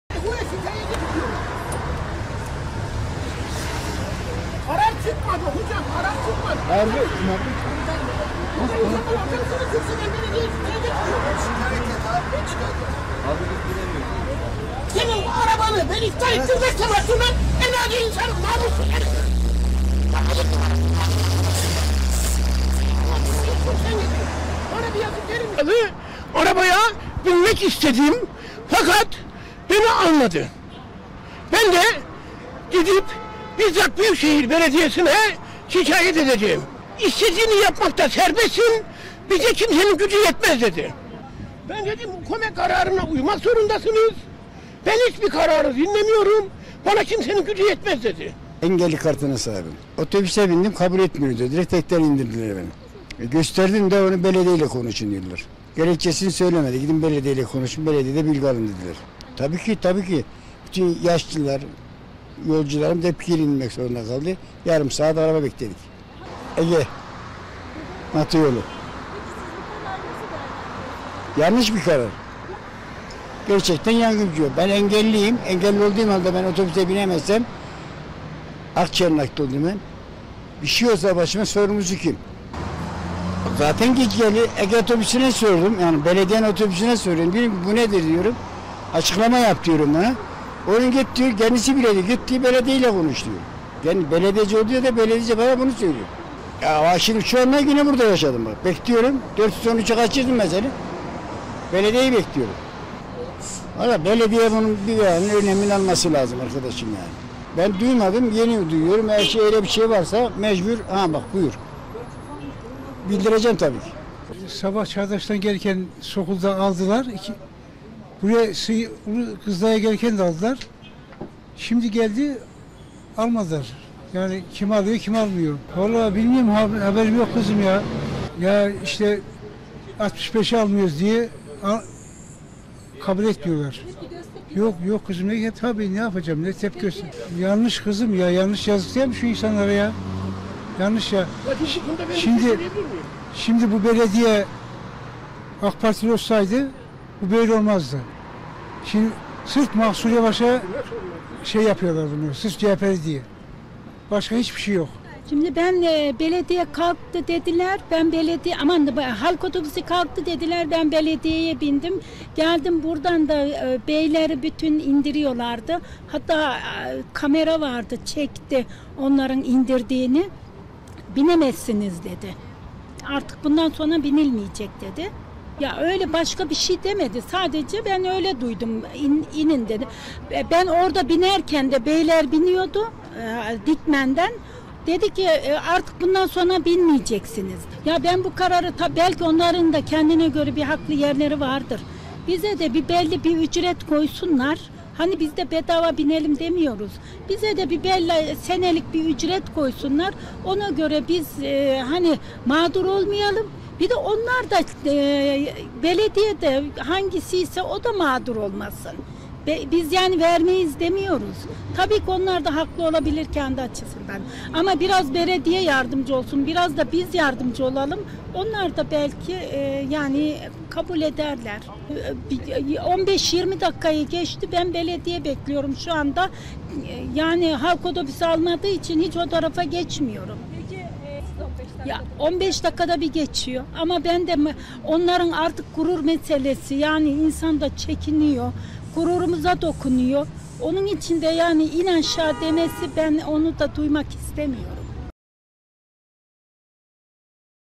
Ankara'da otobüs krizi! "Artık gurur meselesi oldu"
Özel Halk Otobüsleri Birliği, bugün itibariyle artan maliyetleri gerekçe göstererek Ankara’da indirimli ve ücretsiz kart sahiplerini taşımama kararı aldı. Bir otobüs şoförü ile tartışma yaşayan 65 yaş üstü yolcu Murat Ergenekon “Arabaya binmek istedim fakat beni almadı. Ben de gidip bizzat Büyükşehir Belediyesine şikayet edeceğim. İstediğini yapmakta serbestsin bize kimsenin gücü yetmez, dedi” sözleri ile yaşadıklarını aktardı. Bu durumun onur kırıcı olduğunu söyleyen bir kadın da “15-20 dakikayı geçti ben belediye (otobüsü) bekliyorum şu anda halk otobüsü almadığı için hiç o tarafa geçmiyorum. Artık gurur meselesi insan da çekiniyor, gururumuza dokunuyor, onun için de ‘in aşağı’ demesi, ben onu da duymak istemiyorum” dedi.